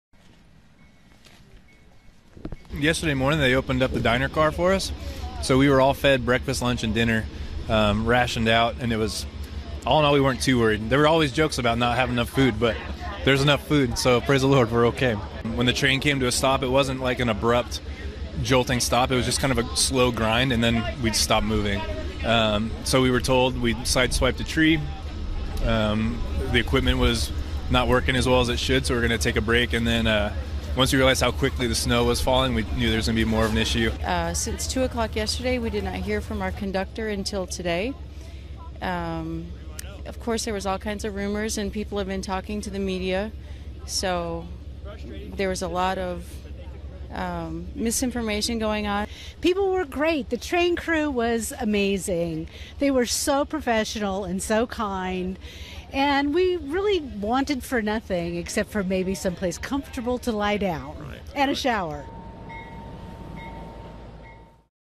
Train Stranded In Oregon Snow With 183 Passengers
February 26, 2019 - An Amtrak train with 183 passengers aboard that had been stuck for at least 36 hours on tracks in the snowy Oregon mountains has arrived at the Eugene train depot. (Feb. 26)